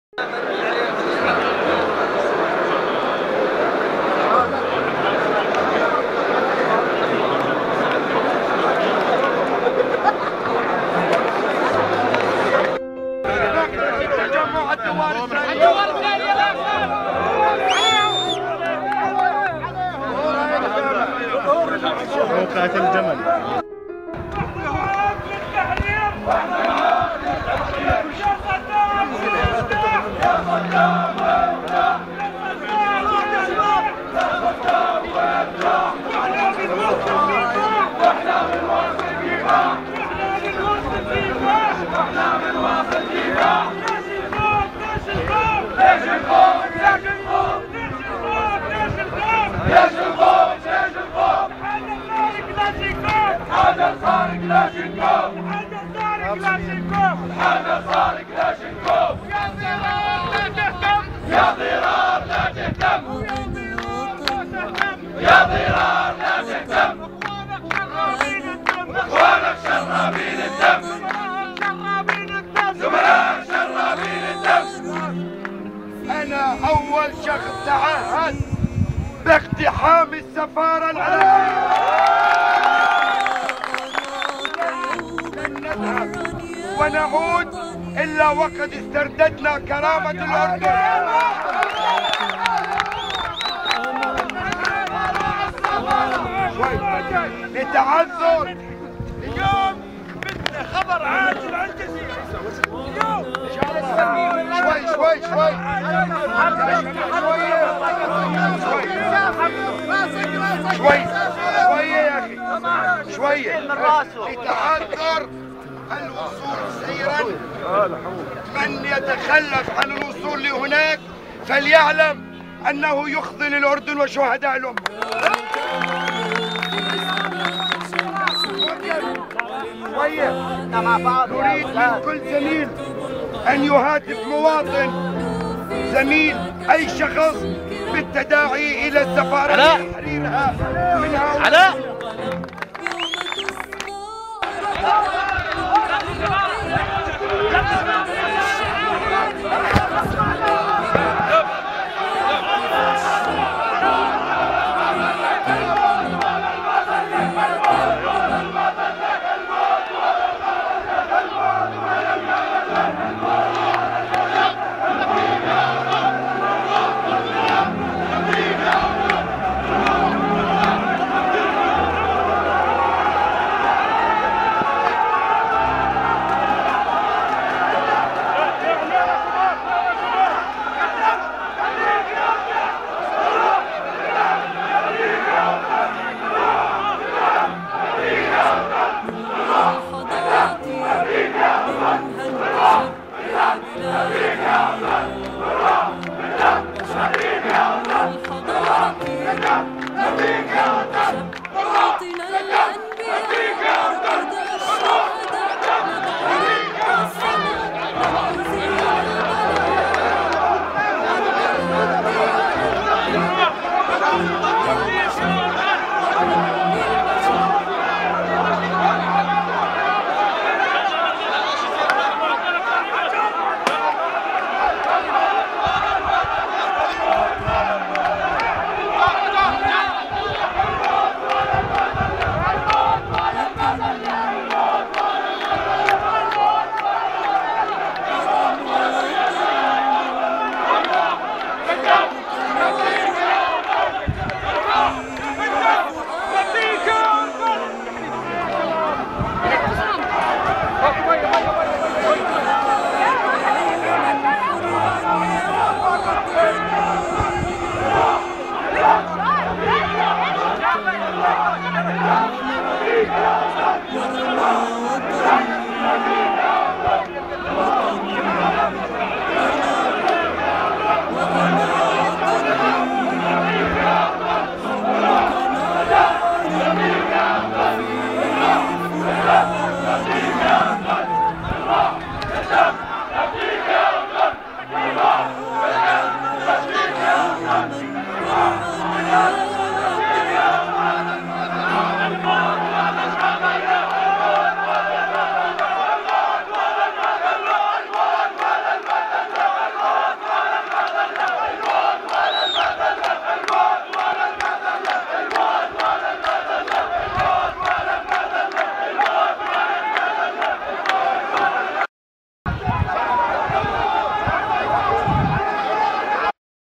حصن العدالة نقابة المحامين تلتف حول اعضائها الذين اعتدي عليهم (2)
تقرير خاص | شبكة قانوني الأردن

حصن العدالة نقابة المحامين تلتف حول الأعضاء فيها الزملاء المحامون زياد النجداوي وضرار الختاتنة ضد الاعتداء الذي تعرضا له في المركز الثقافي الملكي من قبل طاقم السفارة العراقية